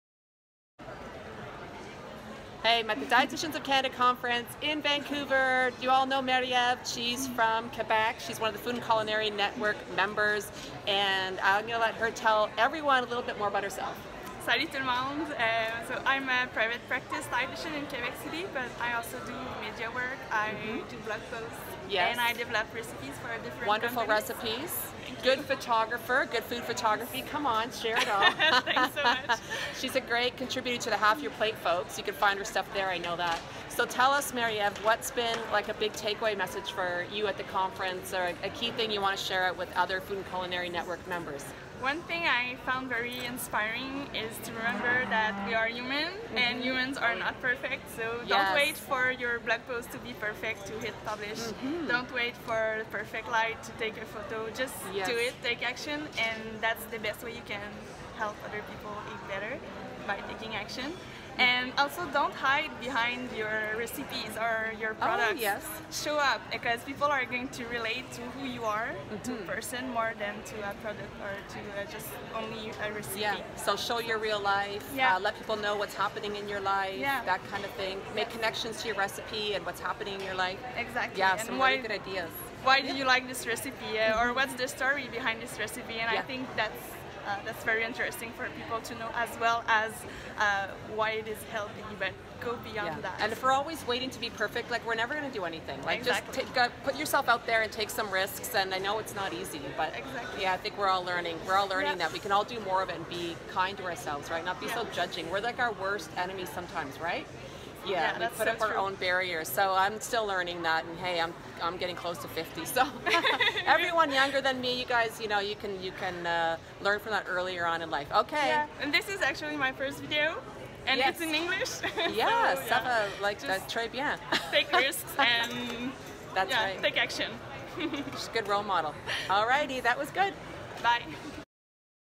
Meet Marie Eve
Marie Eve is a media RD who runs a successful private practice in Quebec. Learn more about the great work she is doing.